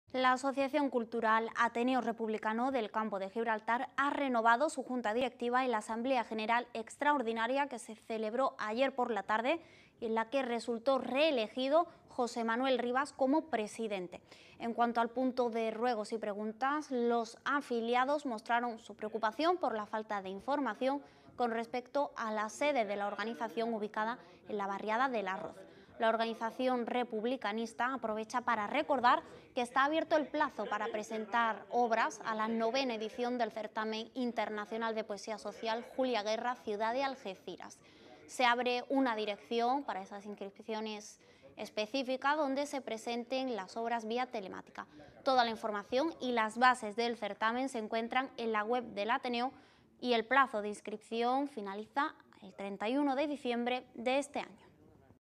La asociación cultural Ateneo Republicano del Campo de Gibraltar renueva Junta Directiva
La  Asociación cultural Ateneo Republicano del Campo de Gibraltar, ha renovado su Junta directiva en la asamblea general extraordinaria que celebró ayer tarde, y en la que resultó reelegido José Manuel Rivas como presidente. En cuanto al punto de ruegos y preguntas los afiliados y las afiliadas, éstos mostraron  preocupación por la falta de información con respecto a las sedes de las organización ubicadas en la barriada del Arroz. La organización republicanista aprovecha para recordar que está abierto el plazo para presentar obras a la IX edición del Certamen Internacional de Poesía Social "Julia Guerra" Ciudad de Algeciras, para la que se abre una dirección de correos específica, donde se presenten las obras, vía telemática. Toda la información y  bases del certamen se encuentran en la Web del Ateneo, cuyo plazo de presentación finaliza el 31 de diciembre de 2017.